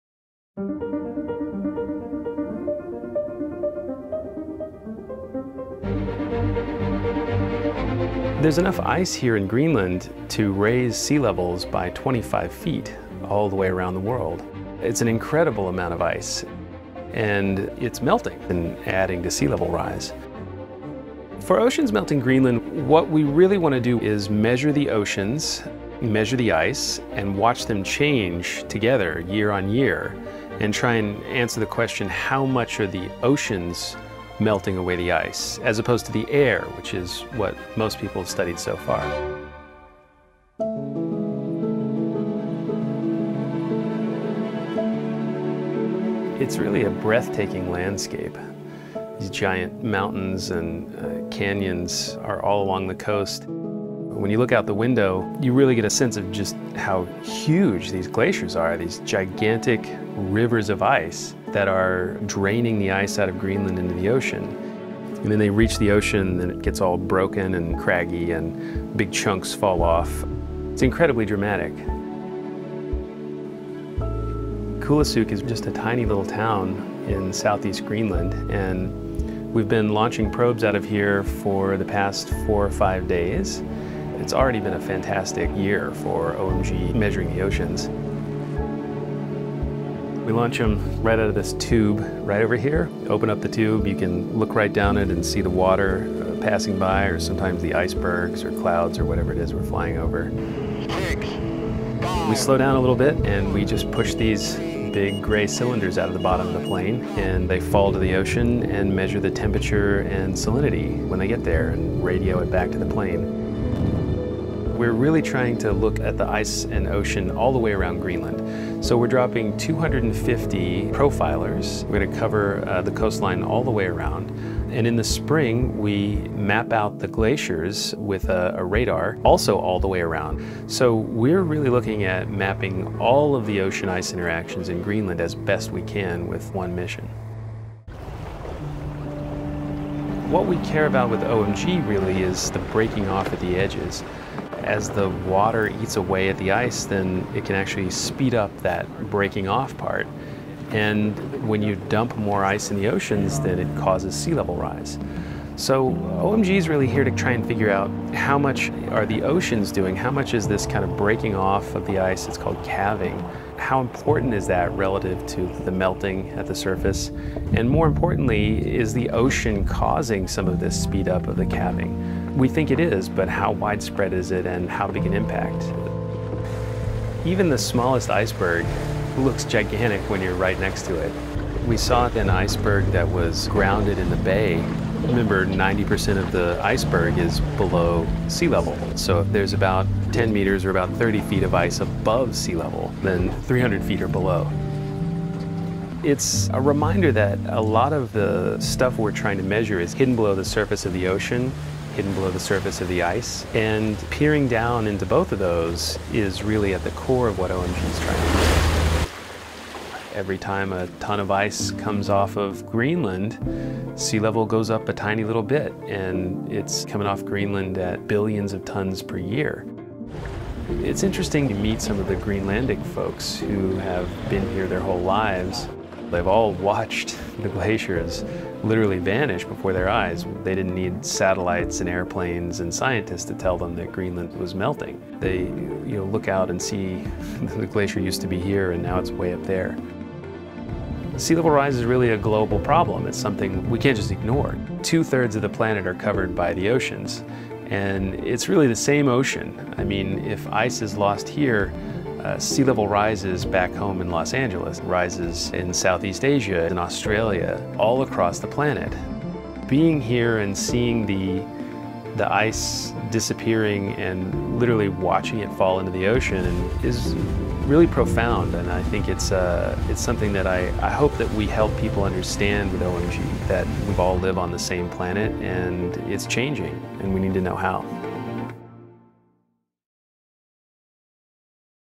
NASA OMG Mission to Study Greenland’s Melting Ice
“There is enough ice here in Greenland to raise sea levels by 25 feet,” says Jet Propulsion Laboratory Scientist Josh Willis in a video about a new NASA mission called Oceans Melting Greenland (OMG). The mission is collecting data  about glaciers and water temperature.

Credit: NASA/JPL-Caltech